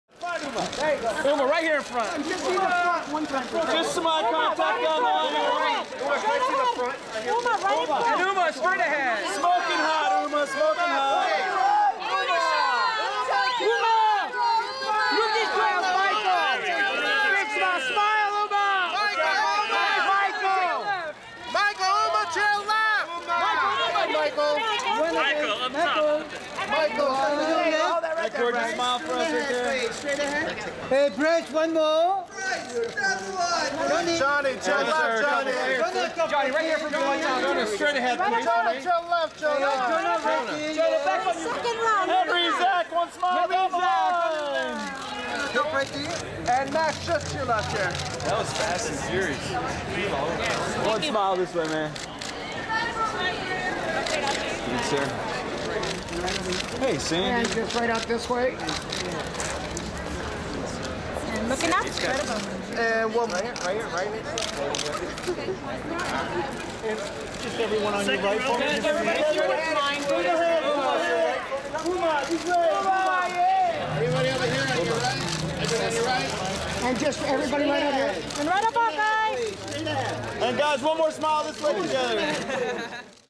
Ceremony Premiere
Coverage of the Ceremony Premiere in Hollywood, CA, on March 22, 2011.